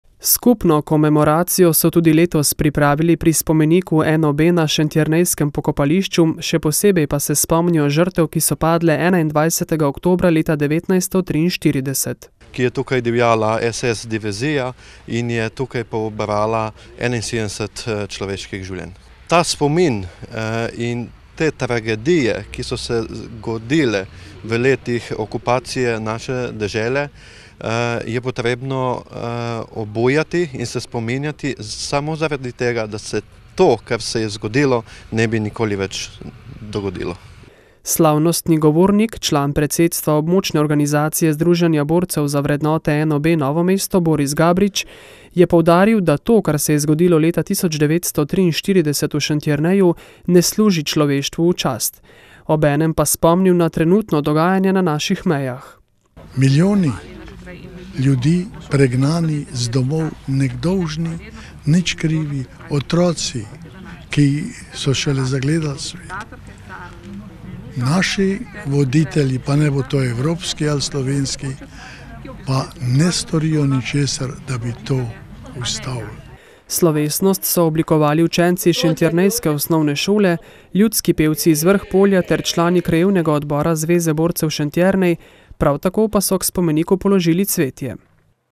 Spomin na padle borce in talce
ŠENTJERNEJ - V šentjernejski občini se v teh spominjajo padlih borcev, pobitih talcev in žrtev bombnih napadov okupatorja v letih druge svetovne vojne